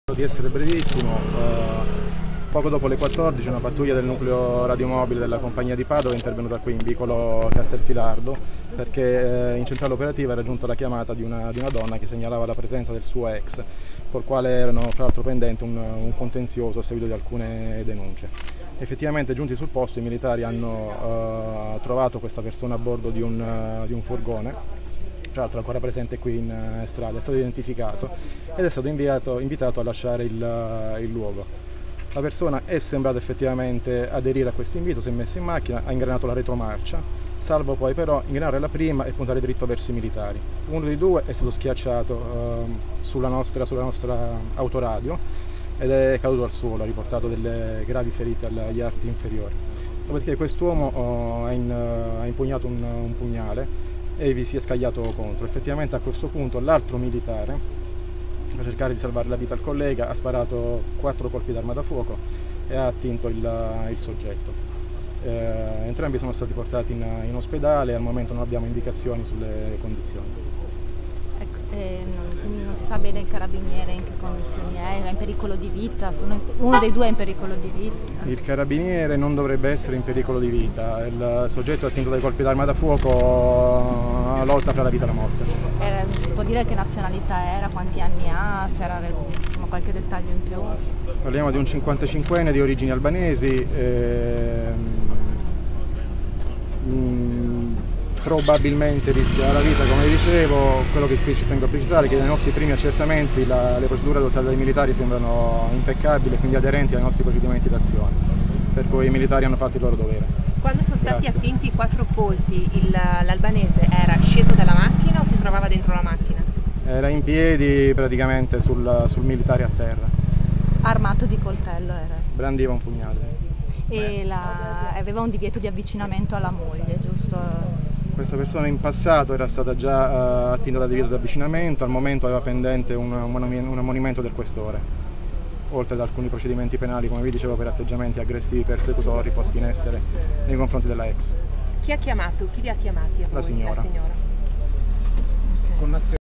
Stalker investe carabiniere, ucciso dai colpi di pistola del collega
Gaetano La Rocca comandante del Reparto operativo dei Carabinieri di Padova ricostruisce la dinamica dell'aggressione ai carabinieri in cui ha perso lo vita un uomo di origini albanesi raggiunto da colpi di arma da fuoco nel pomeriggio del 14 luglio 2023. (video di Silvia Moranduzzo)